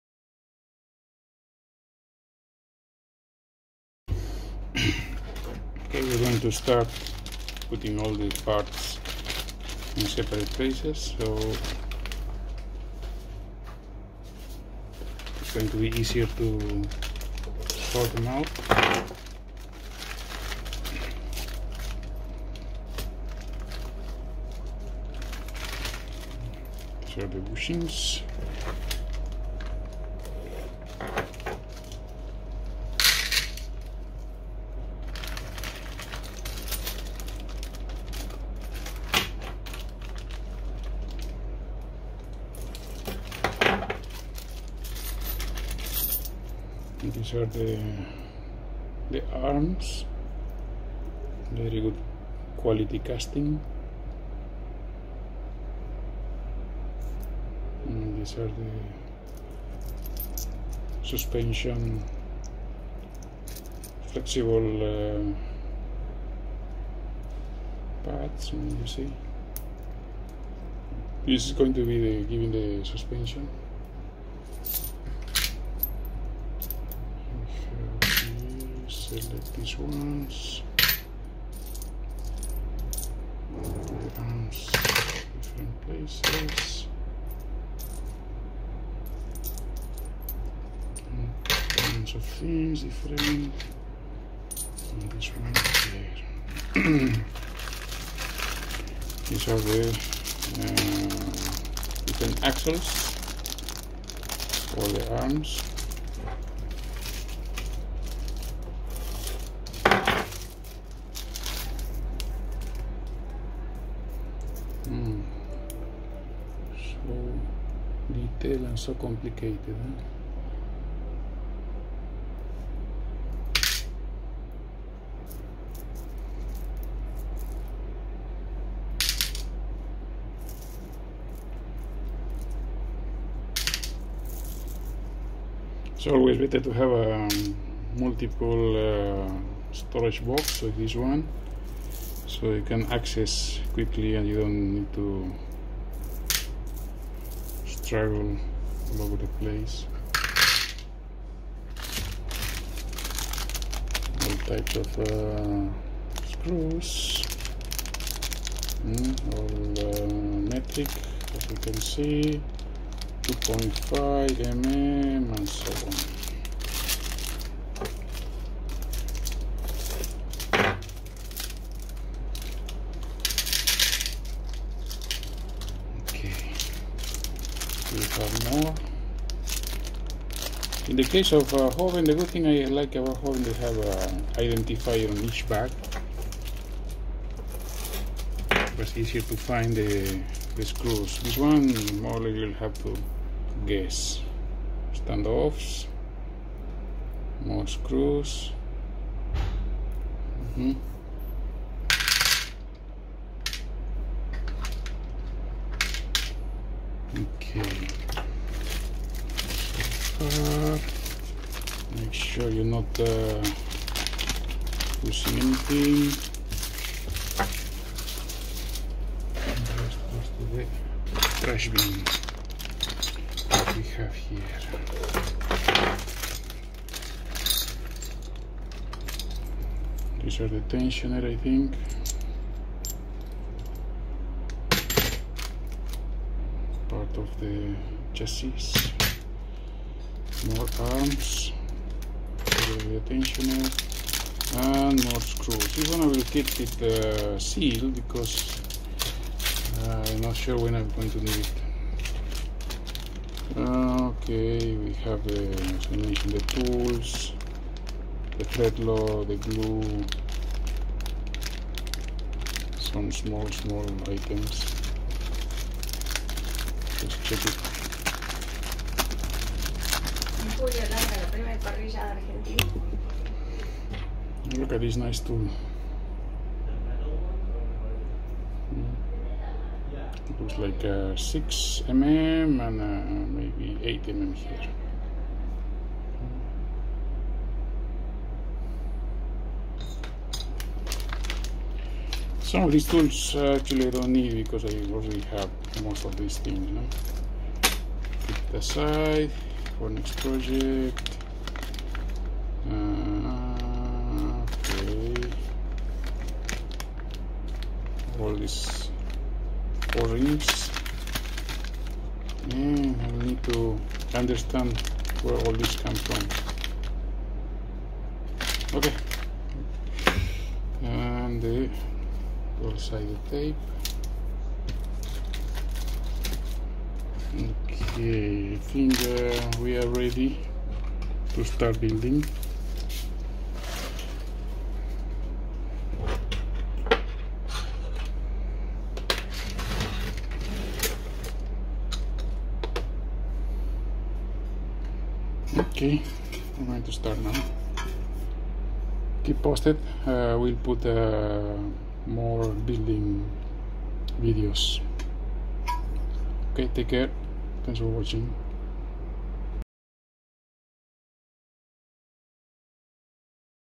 Jgd Pz IV Part #1: Preparation
Order in the workshop and the parts for a smooth build.